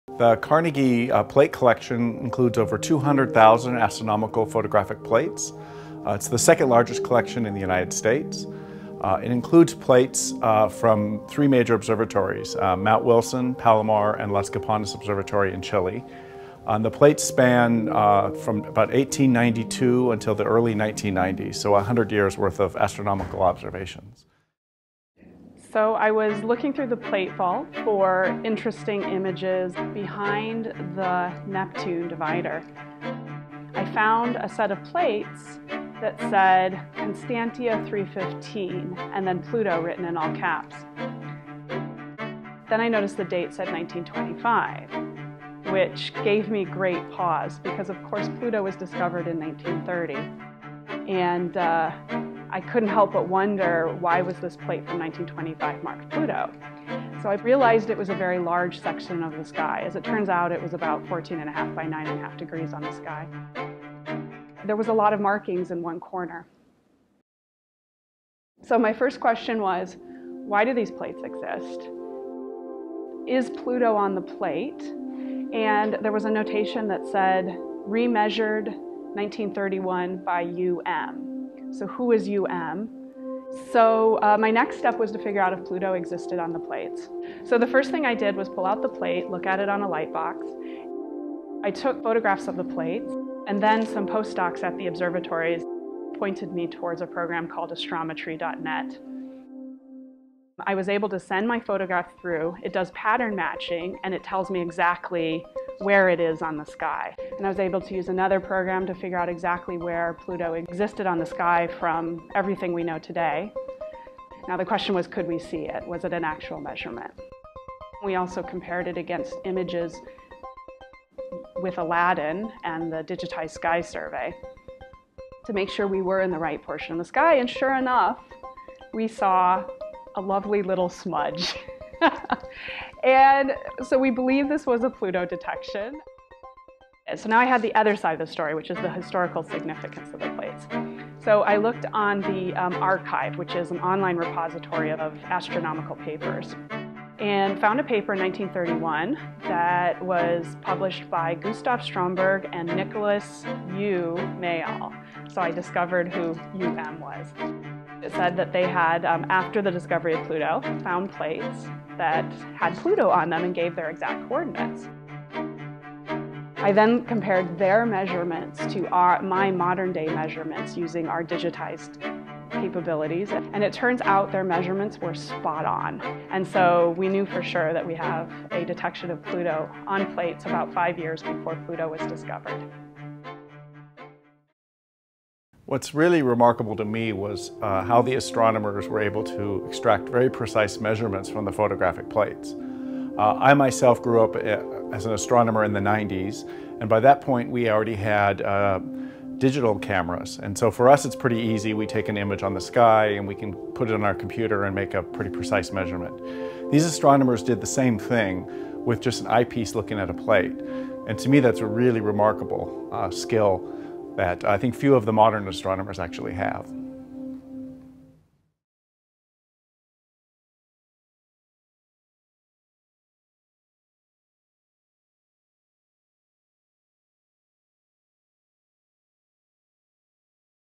Pluto before Pluto
With the New Horizons historic flyby of Pluto next week, imagine how excited we were a few weeks ago to unearth a set of plates from 1925 in our vault that include Pluto--five years before Pluto was discovered. This unexpected find led to a bit of historical detective work to uncover the story of these unusual astronomical plates. The short video tells the tale of our digging, and what we learned from it, and highlights the amazing work done by astronomers of an earlier, pre-digital era. You never know what you might find in your own archives!